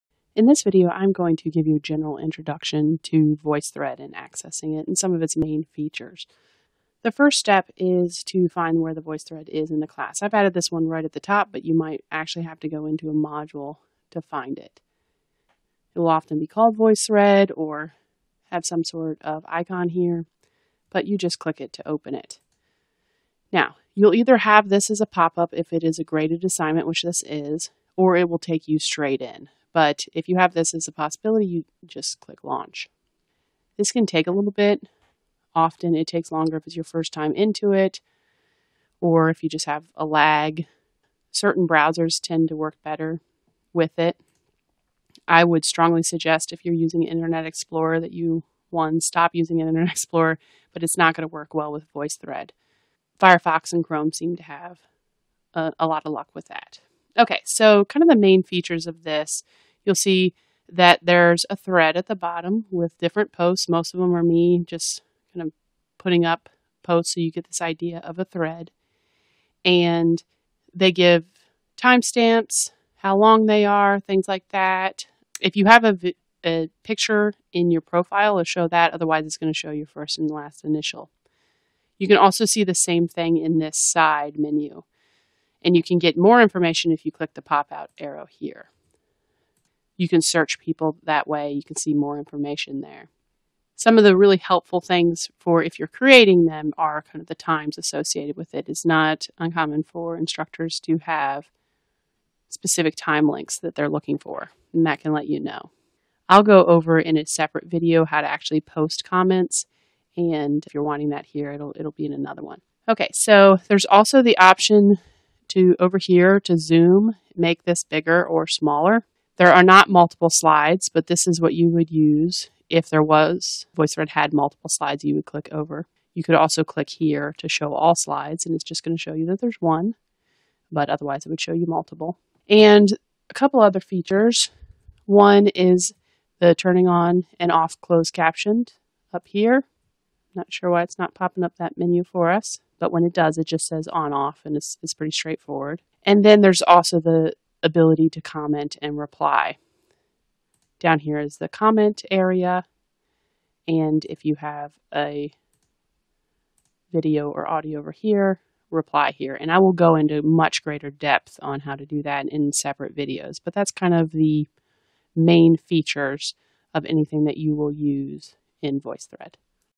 General Overview: VoiceThread Student View
This shows the main features of a VoiceThread from a student's view.